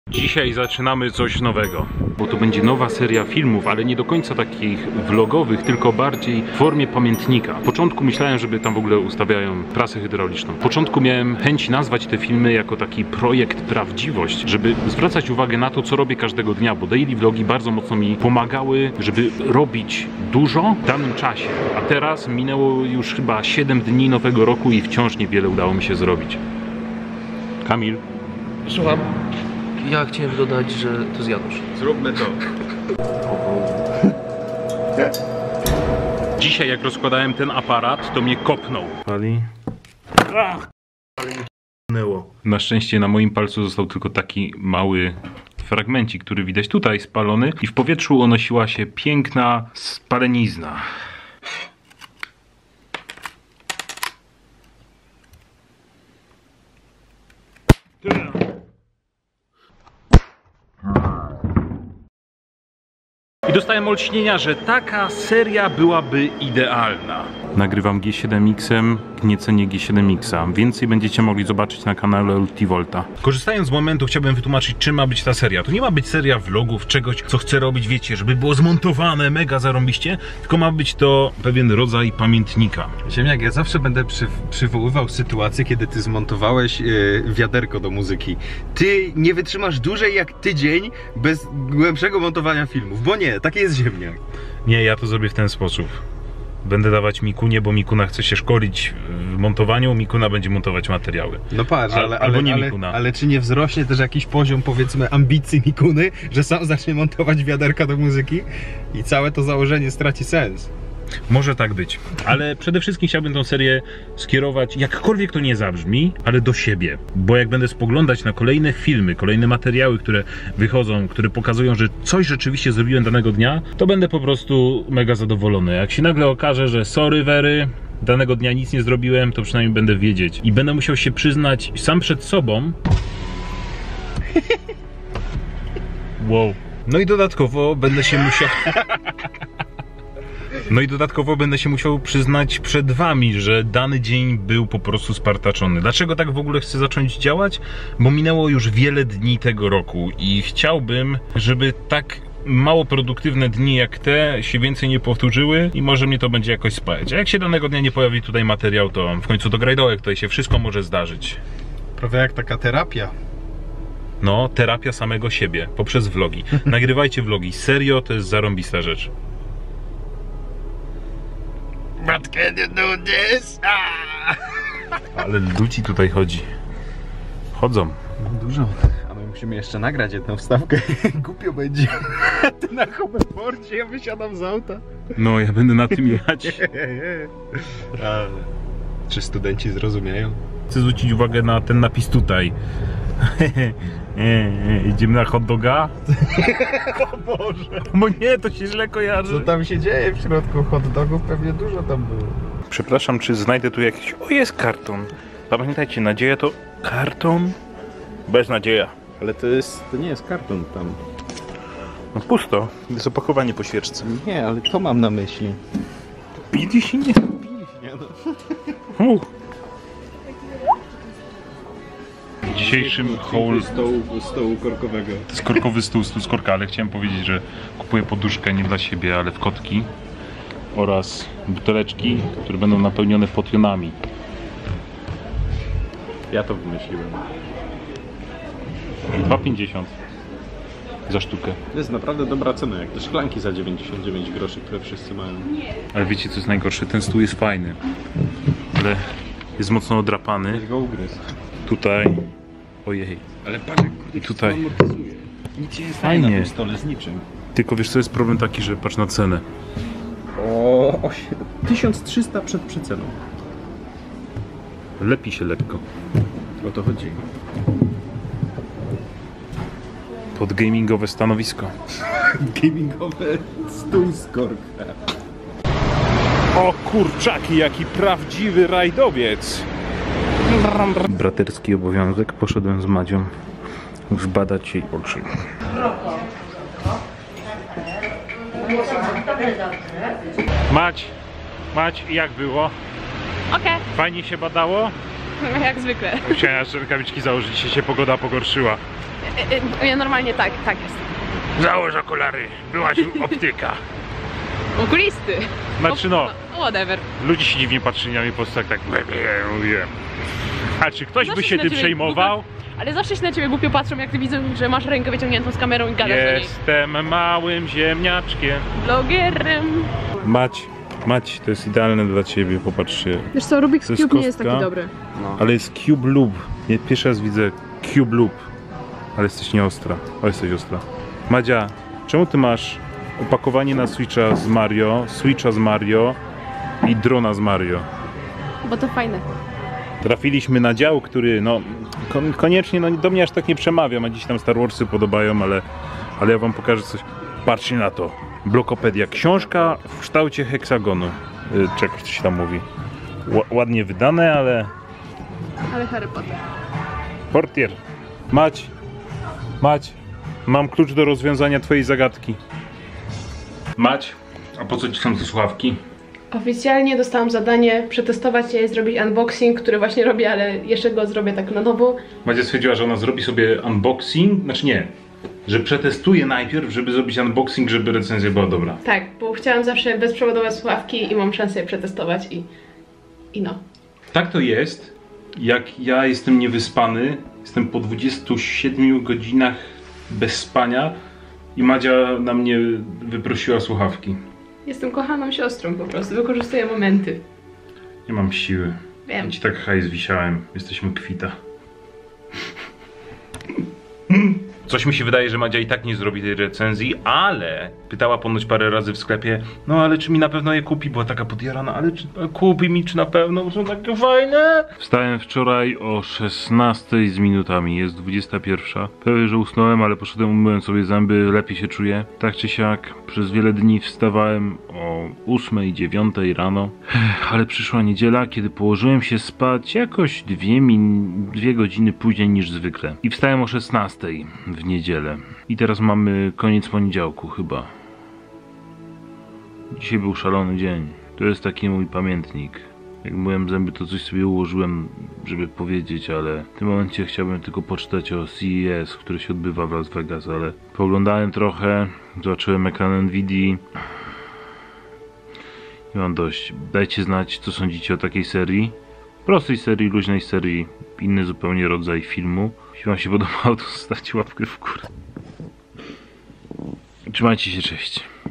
PAMIĘTNIKI Z ZIEMNIAKA #01
­No.. także... ROBIMY TO?

Myślicie, że Mikuna się zgodzi montować?

Ogółem wytłumaczę dlaczego tak, bo nie chcę przerywać tworzenia na główny kanał :o Tam wciąż ja się zajmuję wszystkim :)! Więcej pogadamy w kolejnych produkcjach. Chciałbym żeby się to pojawiało w formie daily o 20:00, czyli mamy poniedziałek i materiał ten pojawi się we wtorek o 20:00 (chyba że znów przerąbię sprawę i zarwę noc, to nie będzie daily vlog, tylko daily wg. dni moich czy coś xD? oczywiście nie liczę przerwy na pisz pisz podczas spania, bo to byłby baaaardzo krótki i nieciekawy odcinek xD)

PYRAX TO DOBRA FIRMA, JEST SUPER.

Wbijajcie także na socialki, takie jak facebook:
Snap - ziemniakization